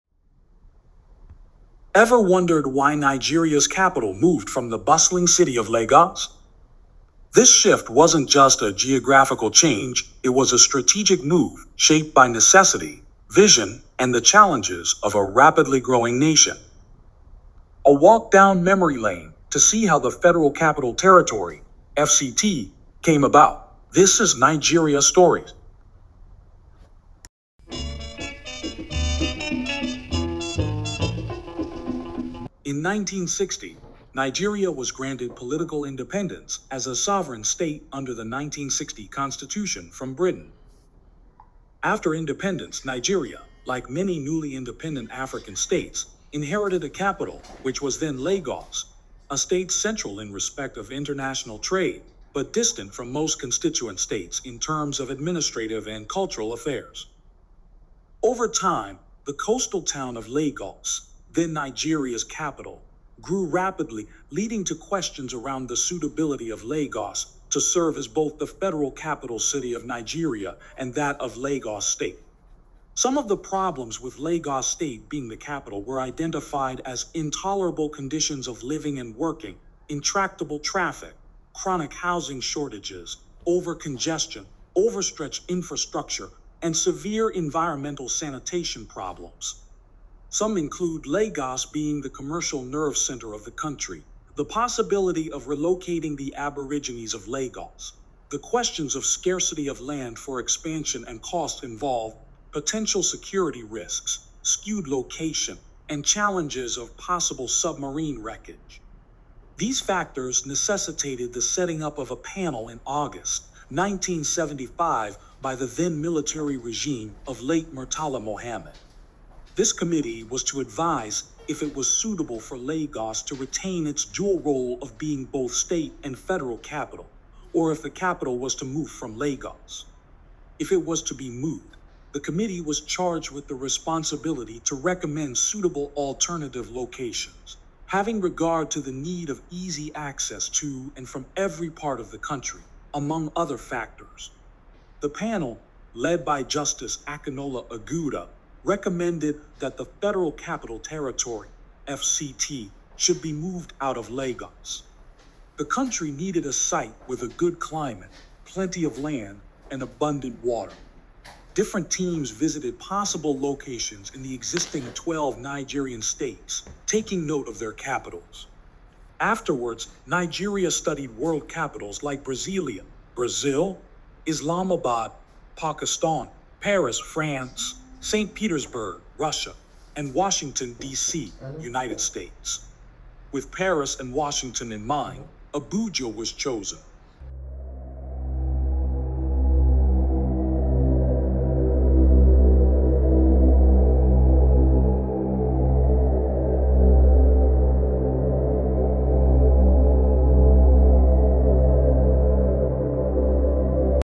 Story of How And Why the Federal Capital OF Nigeria 🇳🇬 Was Moved From Lagos to Abuja.
Footage credit:

France 24
gettyimages
Reuters News Archive
iamgrant_pictures